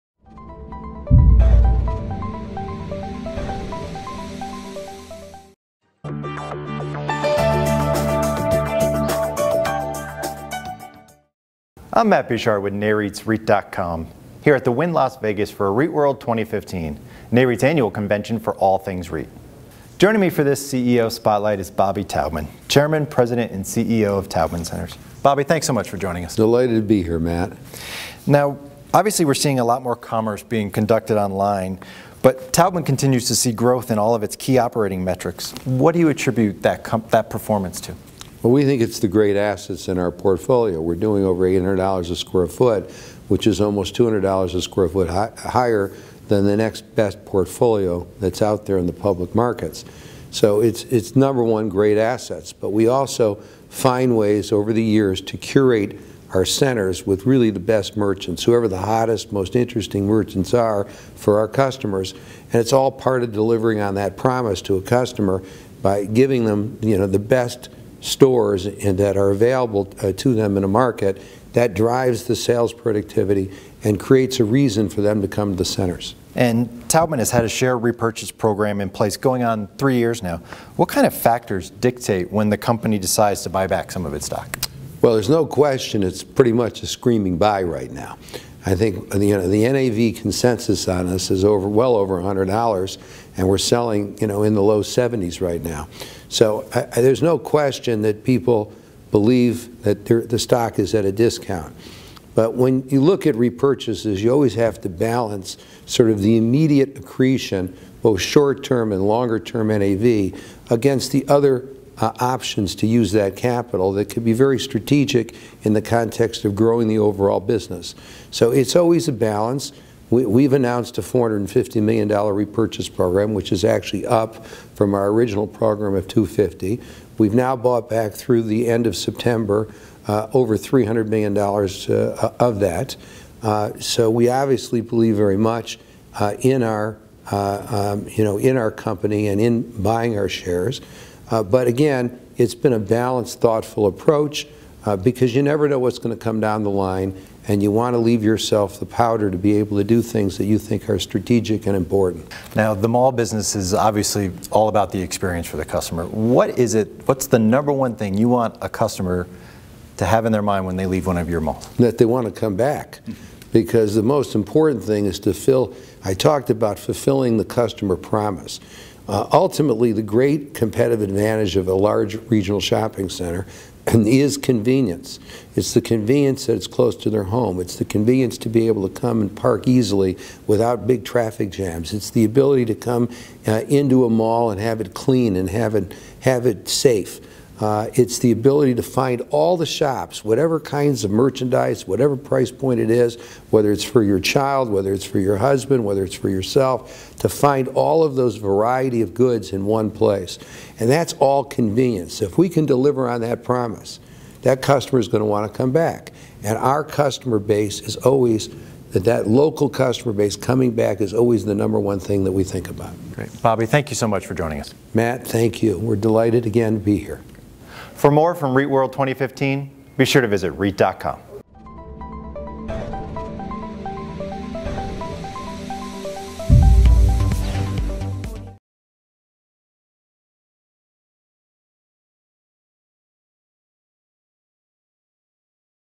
Taubman Says Quality of REIT’s Assets Key to Growth
Bobby Taubman, chairman, president and CEO of Taubman Centers, Inc. (NYSE: TCO), joined REIT.com for a CEO Spotlight video interview at REITWorld 2015: NAREIT’s Annual Convention for All Things REIT at the Wynn Las Vegas.

The REIT owns, manages and leases regional, super-regional and outlet shopping centers in the United States and Asia. Taubman explained that a major factor behind the company’s continued growth in key operating metrics is the quality of its assets.

“We’re doing over $800 (in sales) per square foot, which is almost $200 per square foot higher than the next-best portfolio that’s out there in the public markets,” Taubman said.

The retail REIT’s strong sales record reflects its ability to attract high-quality merchants, creating a compelling reason for customers to come to the centers, according to Taubman.

Taubman also commented on the REIT’s active share repurchase program.

“It’s no question it’s pretty much a screaming buy right now,” Taubman commented in reference to the current share price. However, before making a share repurchase, the REIT has to weigh the move against using its capital for other objectives, he added.

The company has announced a $450 million share repurchase program and has bought back more than $300 million in company stock through the end of September.  “We obviously believe very much in our company, but it’s been a balanced, thoughtful approach,” Taubman said.

12/8/2015 | By Sarah Borchersen-Keto